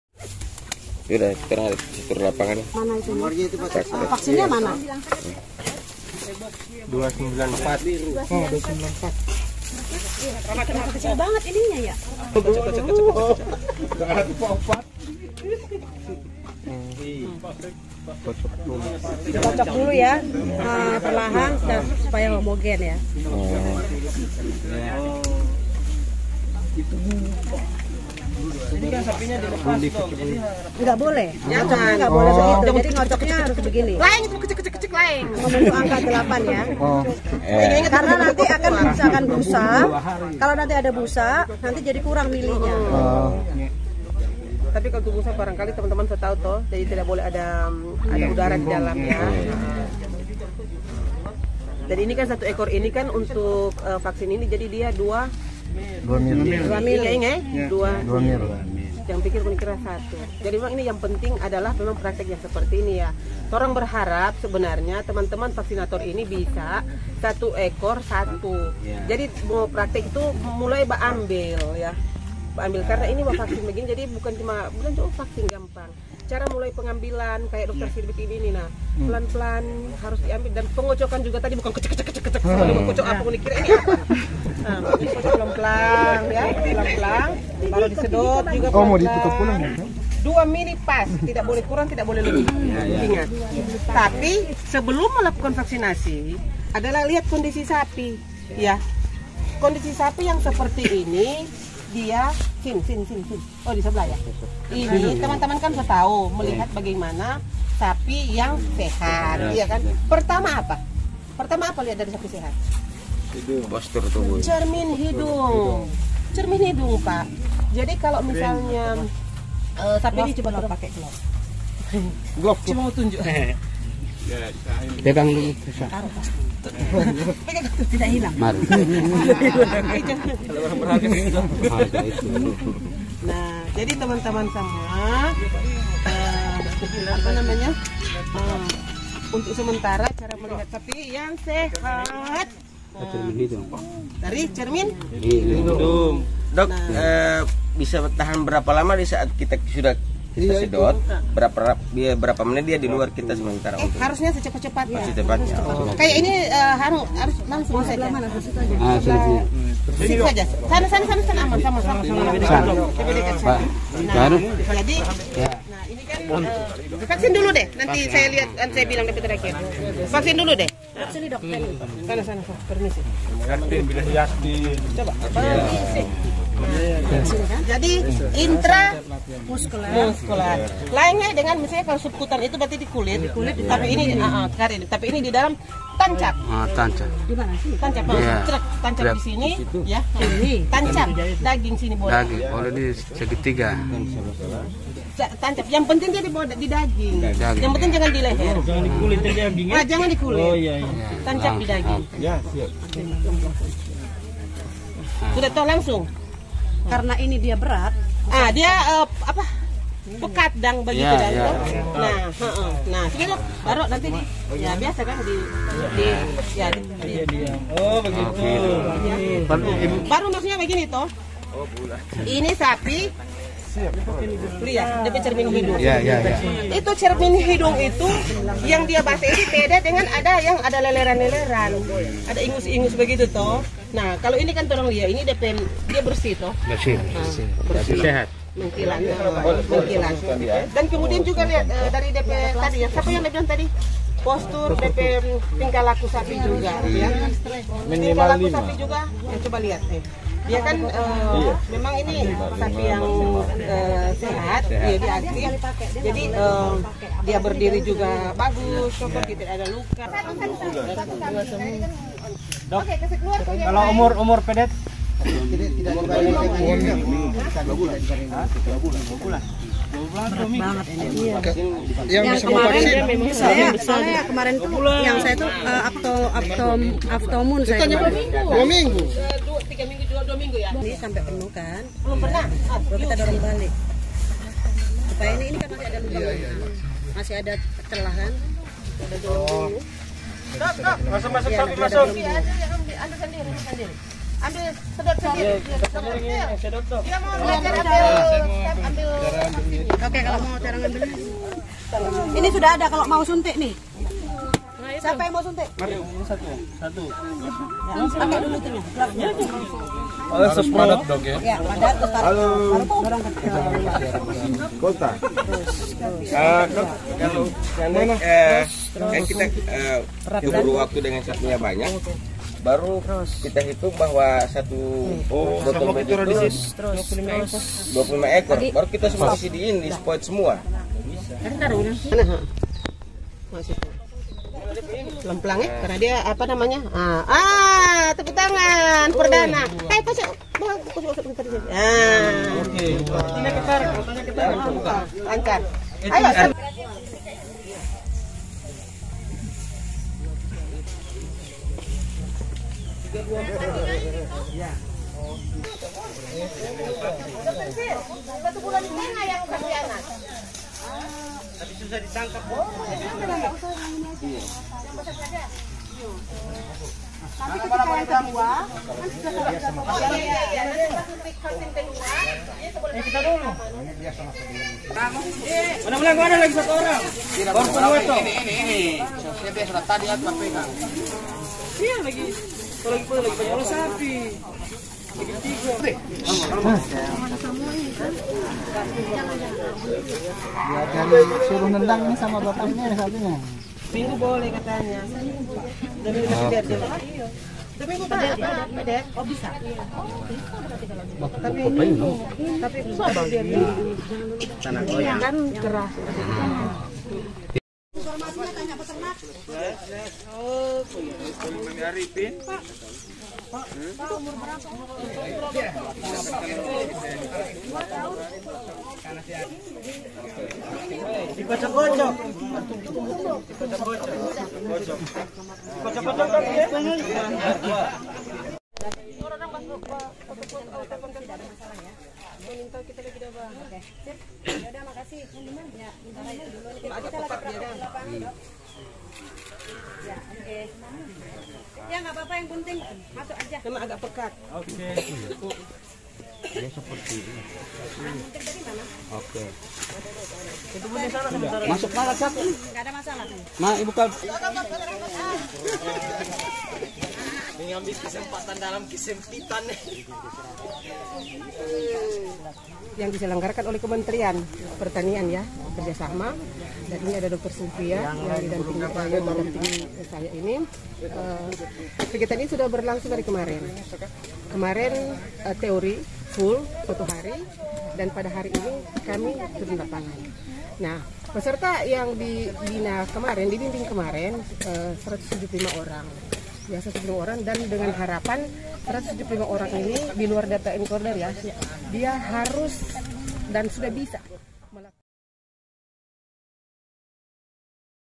Praktek Vaksinasi PMK Oleh Dokter Hewan Cantik
Praktek vaksinasi PMK oleh dokter hewan kepada peserta bimtek